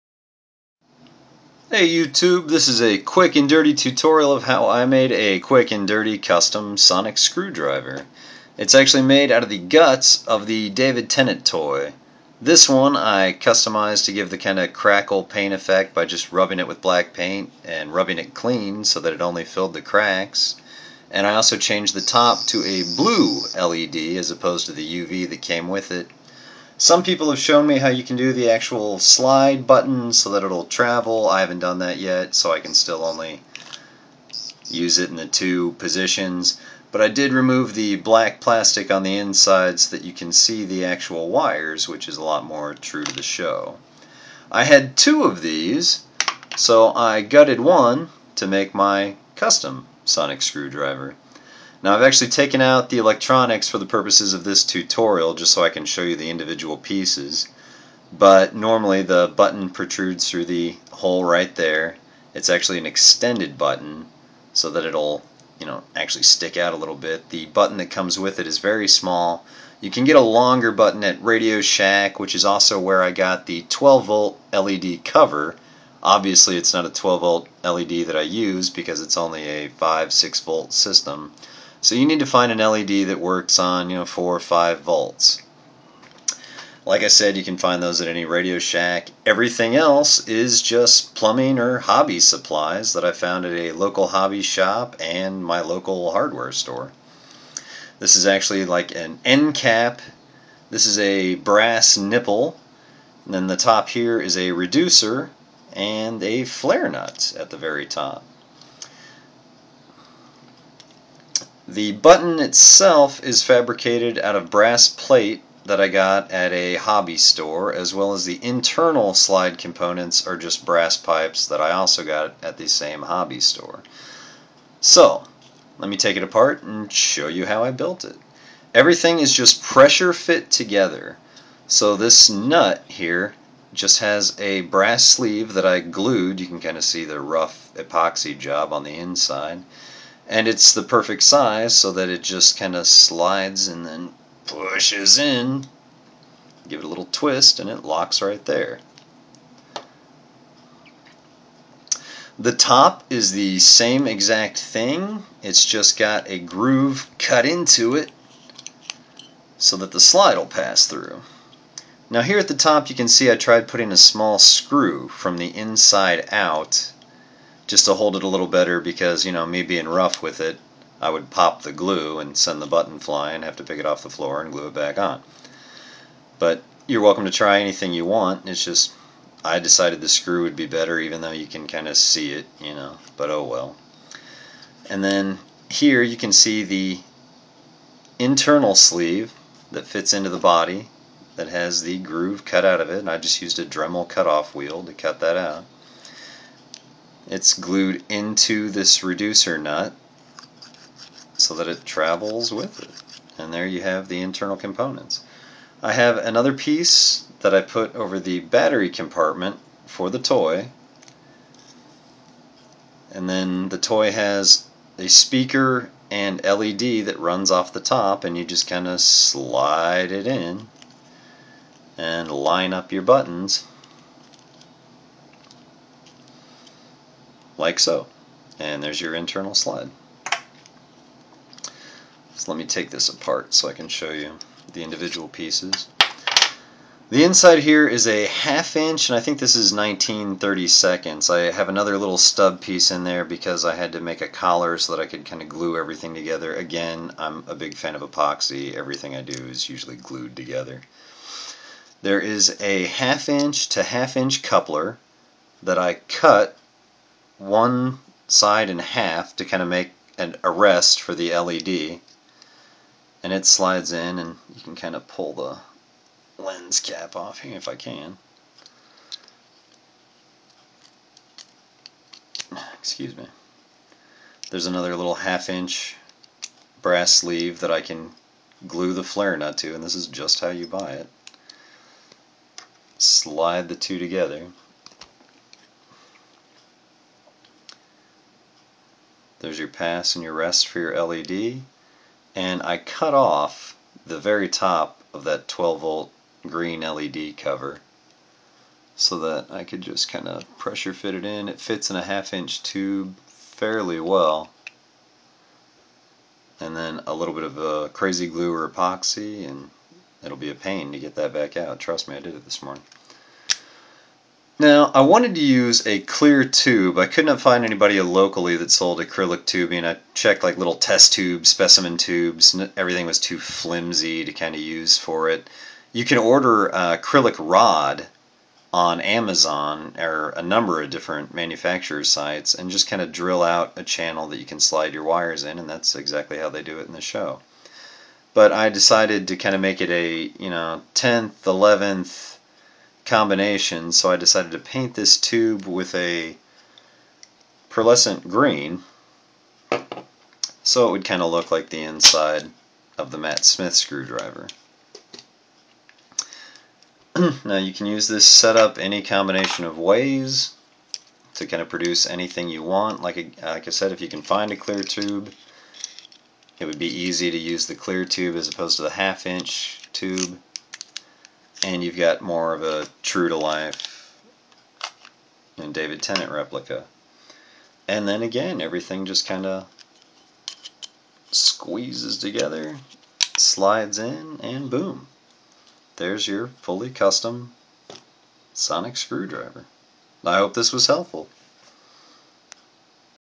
Custom Sonic Screwdriver Mk I Tutorial
Made with 40 bucks and some blood, sweat, and tears. Easy to do if you use the electronics from one of the toy sonics. This is a simple solution to a custom sonic if you don't have access to a well equipped machine shop.